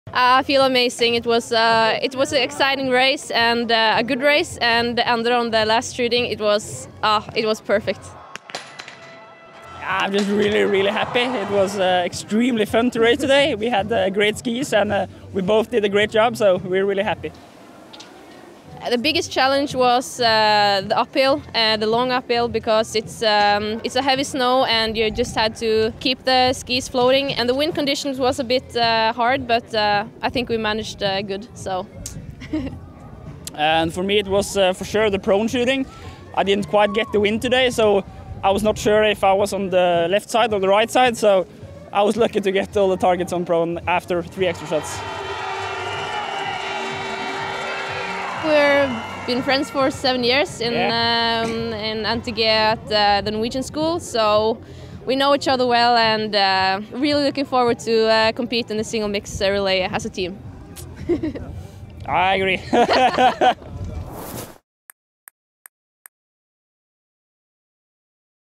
Norway claims the Single Mixed Relay in Minsk-Raubichi
Karoline Erdal and IBU CupTotal Score leader Endre Stroemsheim claimed the Gold Medal for Norway in the Single Mixed Relay at the IBU Open European Championships.